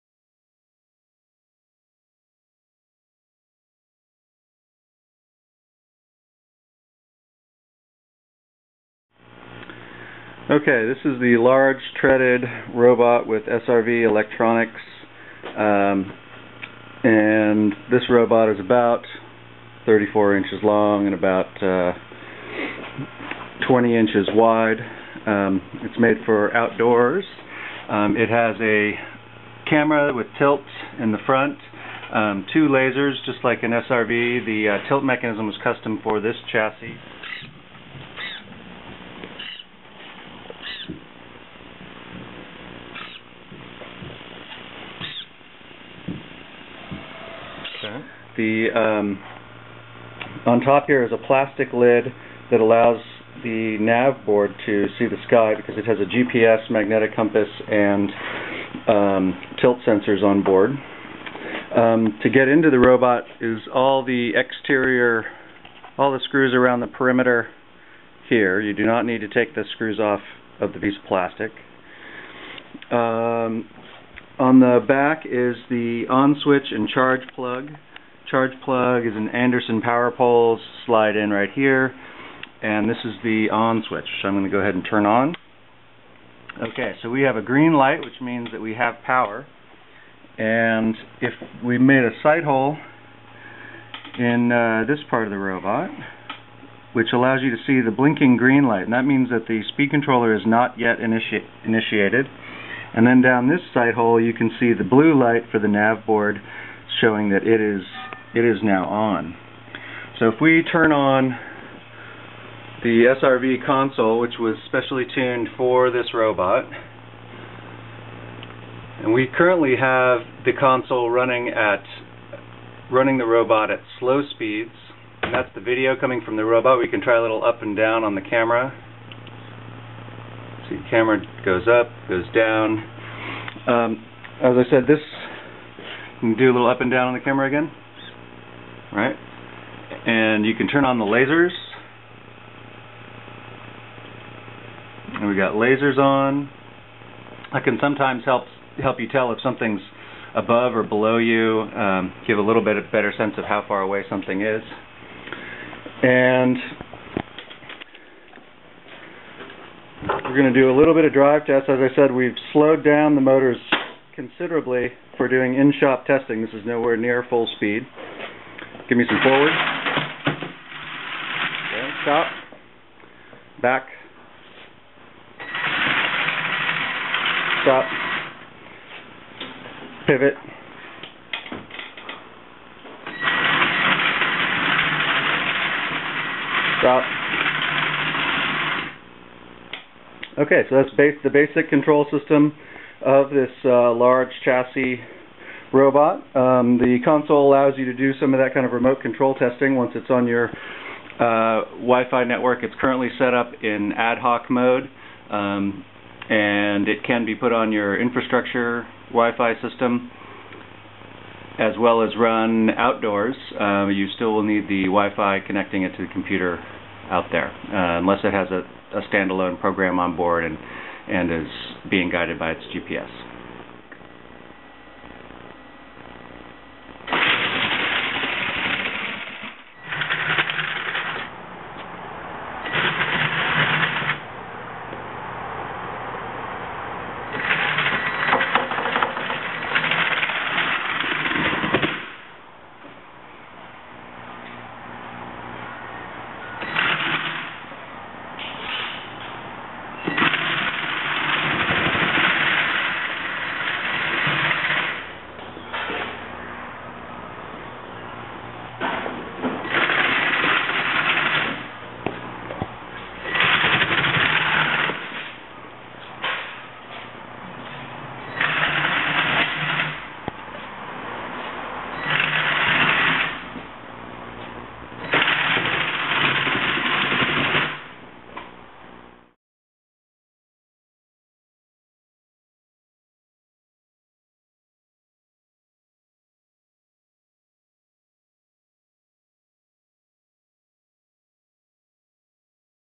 Treaded Robot, Large open source Surveyor Robot with GPS
This large scale outdoor robot with Surveyor SRV electronics was built for one of our clients.  It uses four gear gear motors, a separate speed controller (from the usual built in SRV version), camera tilt servo, and the navigation board (GPS, tilt sensors, and compass).  We can build this in a few sizes, also with wheels instead of treads, this one is the largest off the shelf chassis option.